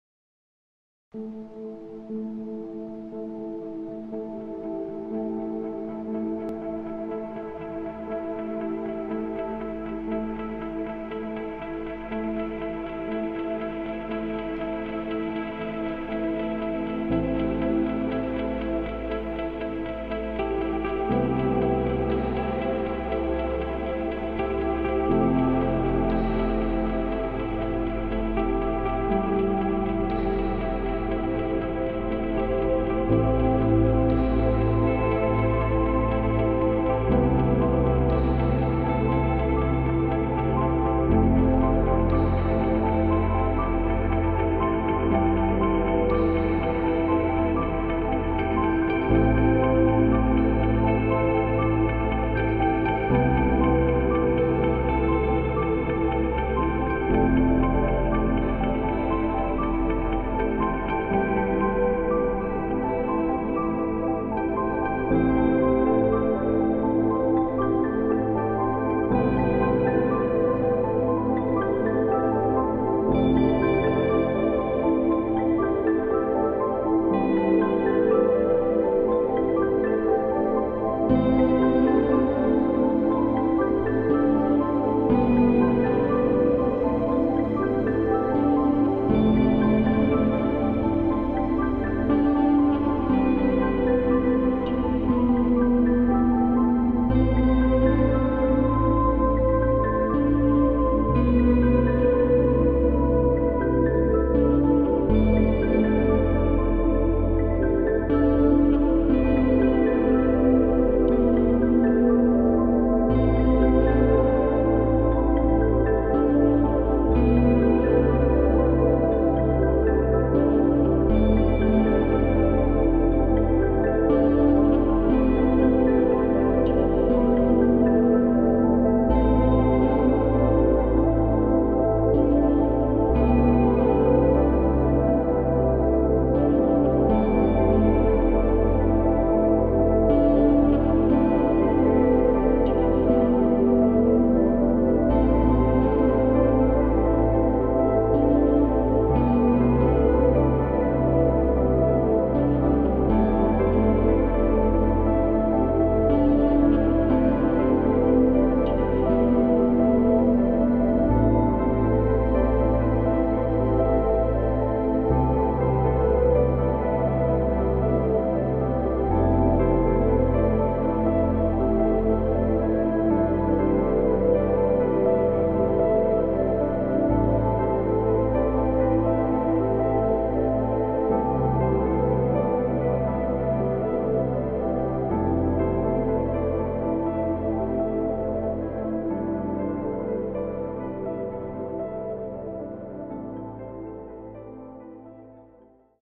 Tour of a 2019 Porsche Macan Sport Design | For Sale
As good as new 2019 Porsche Macan Sport Design, now available at our Car Store!